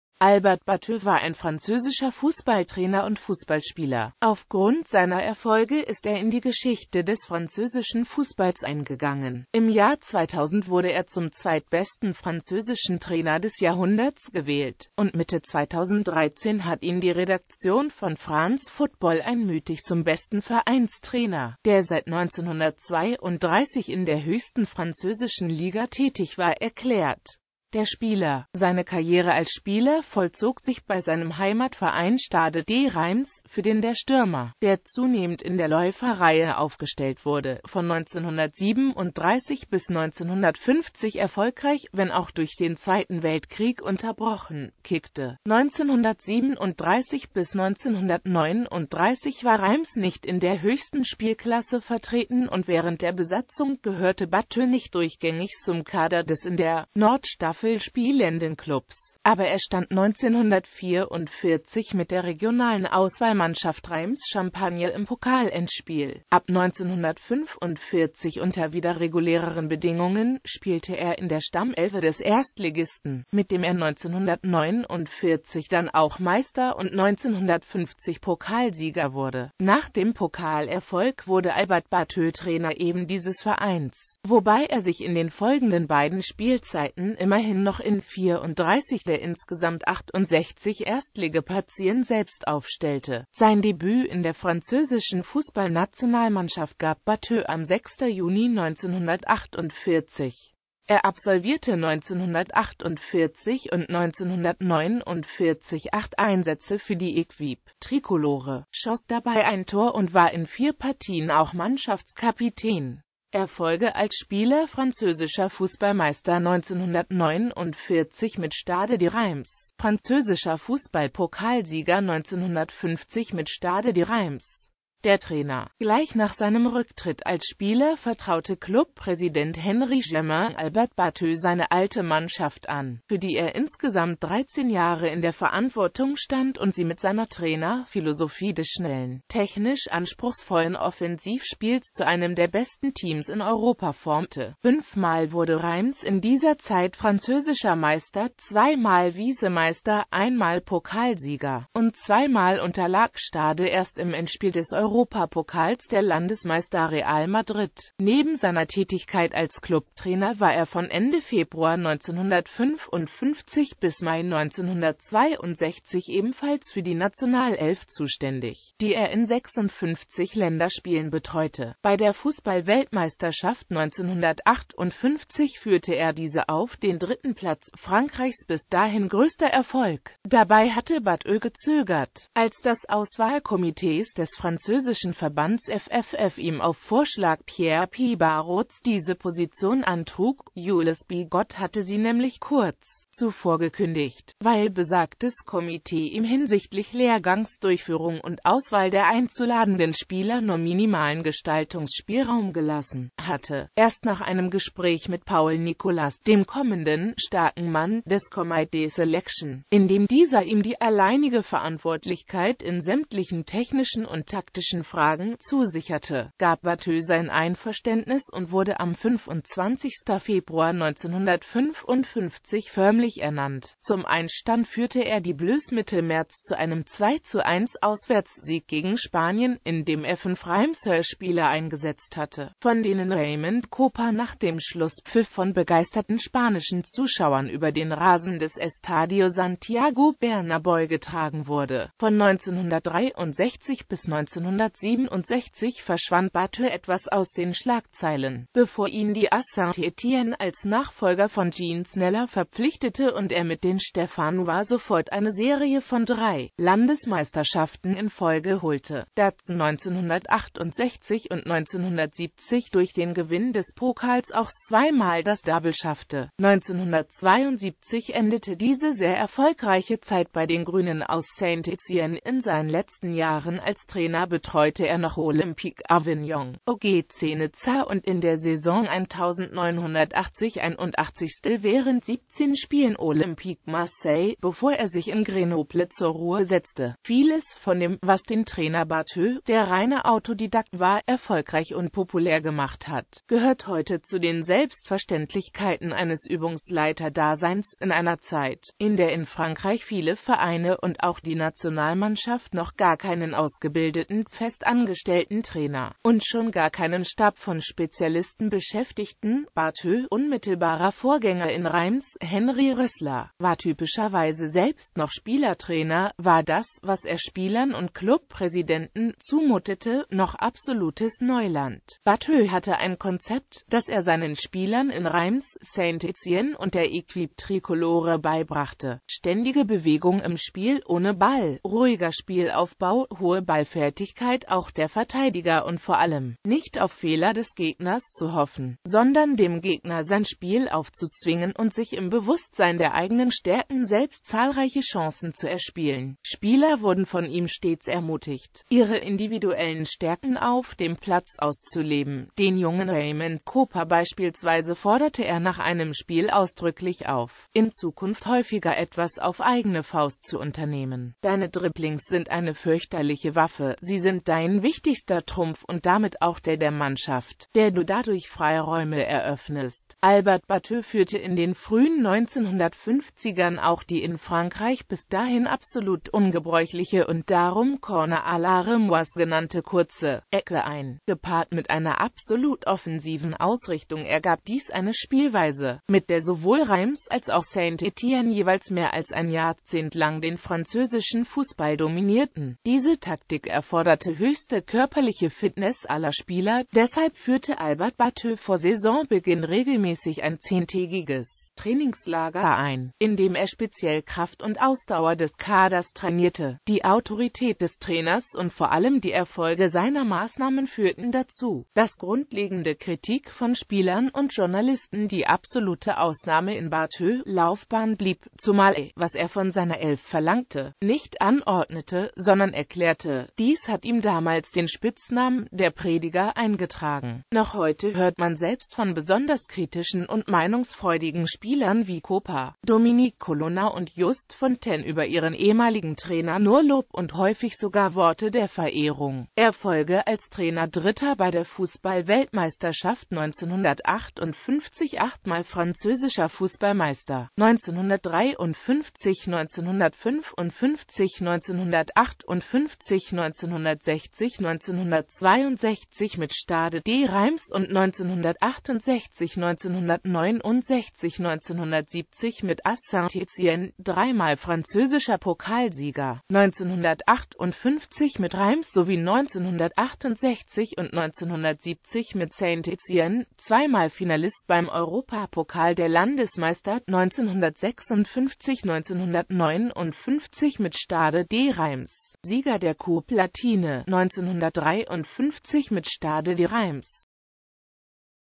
Albert Batteux
Vielen Dank für Ihre Unterstützung:

Albert Batteux war ein französischer Fußballtrainer und Fußballspieler.Aufgrund seiner Erfolge ist er in die Geschichte des französischen Fußballs eingegangen: Im Jahr 2000 wurde er zum zweitbesten französischen Trainer des Jahrhunderts gewählt, und Mitte 2013 hat ihn die Redaktion von France Football einmütig zum besten Vereinstrainer, der seit 1932 in der höchsten französischen Liga tätig war, erklärt.